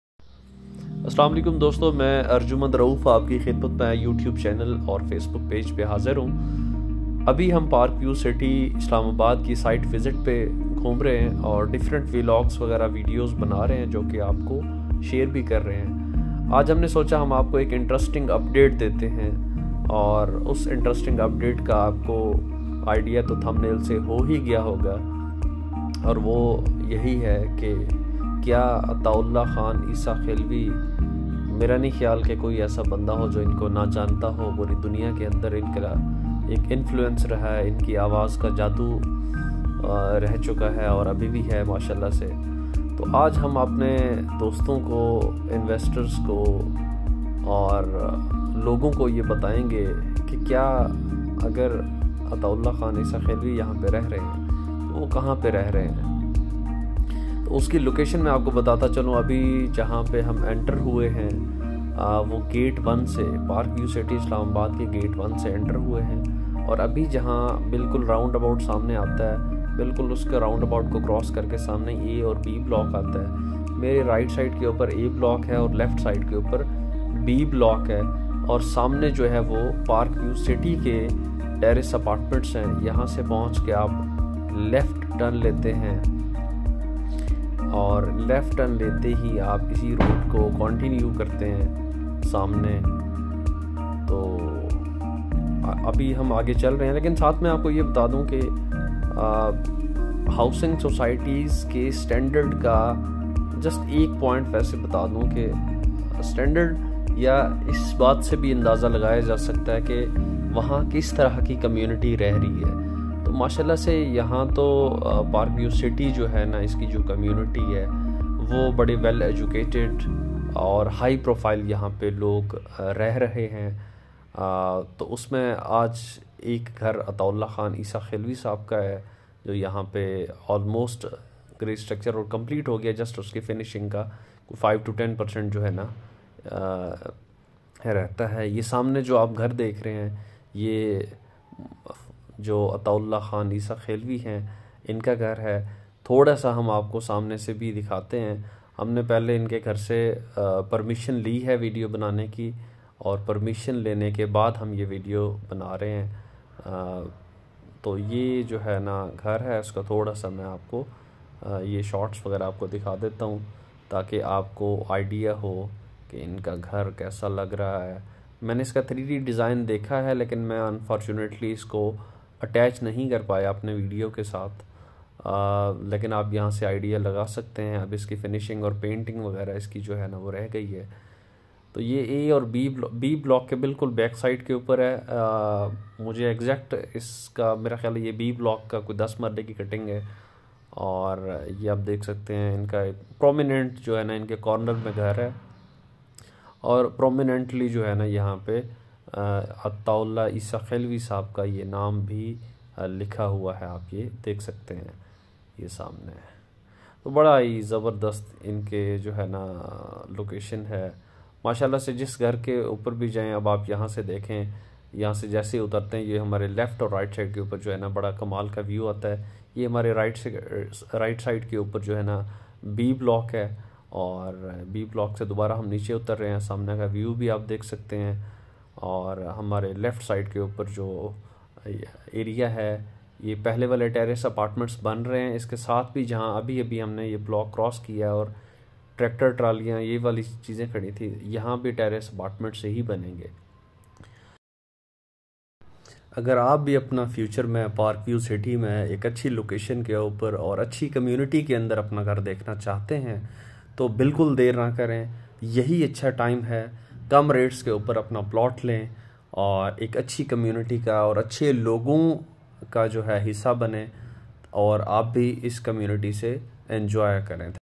Stunning House of Attaullah Khan Esakhelvi in Park View City Islamabad
In this video, we take you on an exclusive tour to the luxurious home of the legendary Pakistani singer Attaullah Khan Esakhelvi, located in the picturesque Park View City neighborhood of Islamabad. From the grand entrance, every corner of this house is a reflection of the singer's impeccable taste and style.

As we walk through the spacious living areas, you'll get a glimpse of the stunning artwork, intricate furniture pieces, and the vibrant color schemes that make this house truly unique. We'll also take you to the singer's private studio where he spends countless hours perfecting his craft.

Join us for this behind-the-scenes look at the lavish lifestyle of one of Pakistan's most beloved artists. Whether you're a fan of Attaullah Khan Esakhelvi's music or simply appreciate elegant design, this video is sure to leave you inspired.